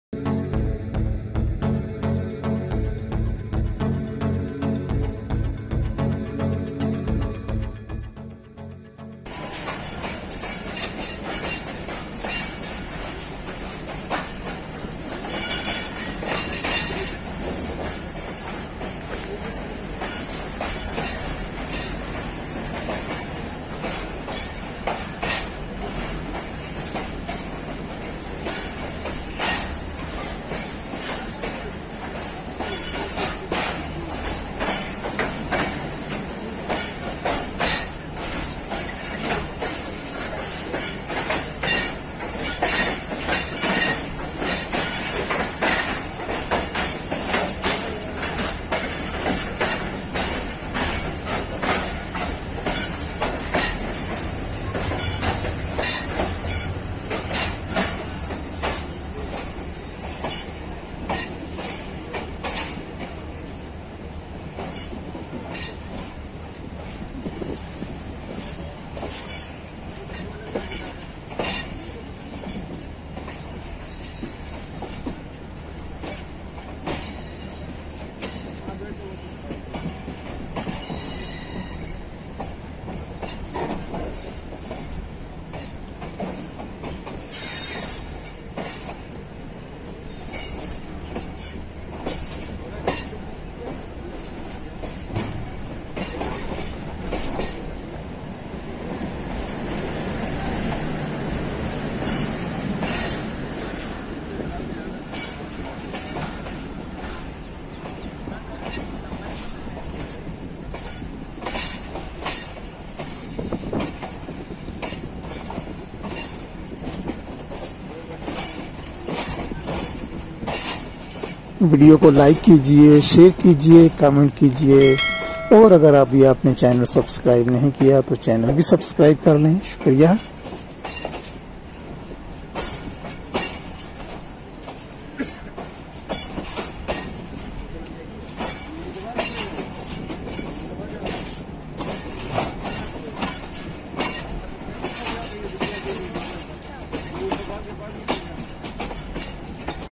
8DN Tezgam Departing Hyderabad Junction Sind Pakistan | Pakistan Railways | Travel Pakistan
8DN Tezgam Departing Hyderabad Junction in Sind Pakistan. It runs between Rawalpindi and Karachi cantt on daily basis.